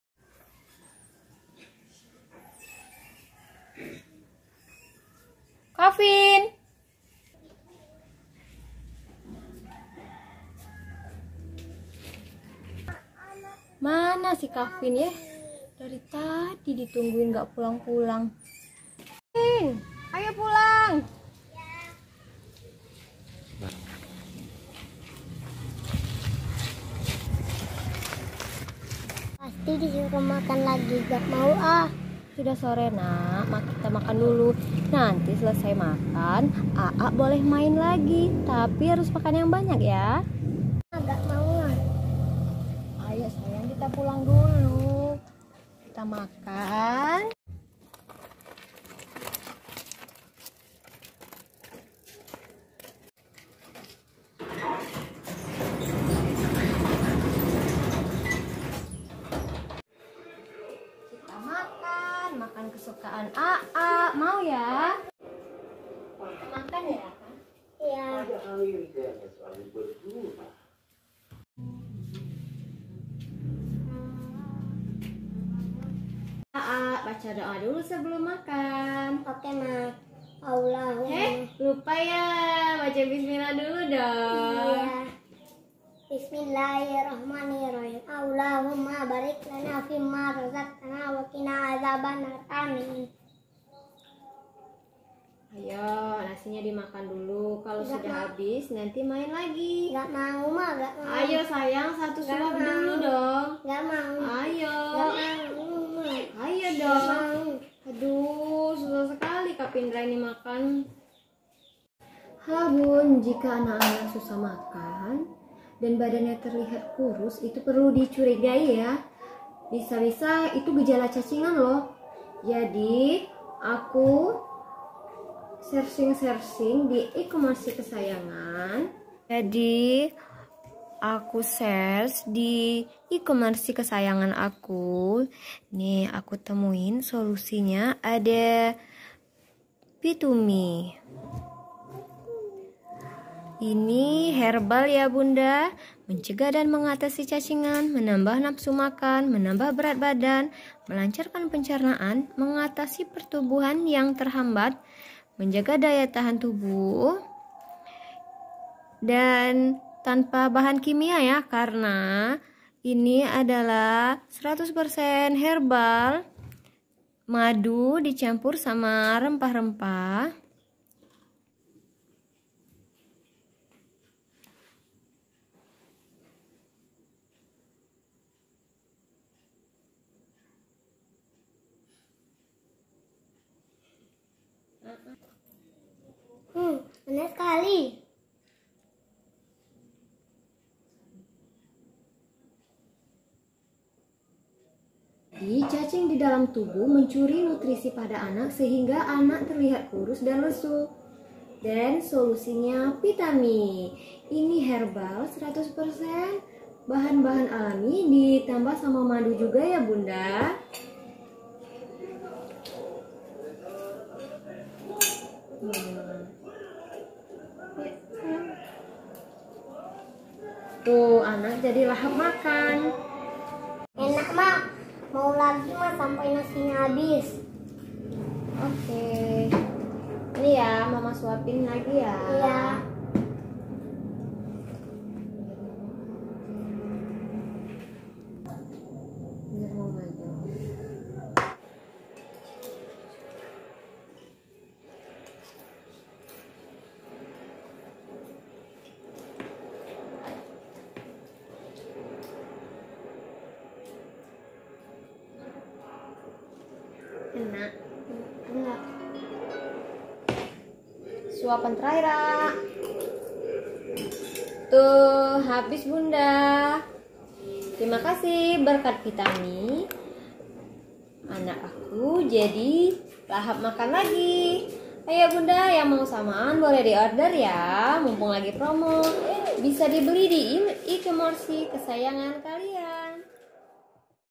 BERKAT MADU VITUMMY ANAKKU JADI LAHAP MAKAN DAN BERAT BADANNYA NAIK
Halo bunda.. Pernah ngga sih bunda merasa khawatir kalau si kecil lemas dan lesu, kurang bersemangat, nggak
nafsu makan, dan berat badannya rendah? Bisa jadi itu gejala si kecil terinfeksi cacing bun..

Manfaat Vitummy :
- Mencegah dan mengatasi infeksi cacing pada anak
- Mengatasi kurangnya nafsu makan
- Menambah berat badan
- Mengatasi masalah pencernaan seperti sembelit,
diare dll
- Menjaga daya tahan tubuh
- Mengatasi pertumbuhan yang terhambat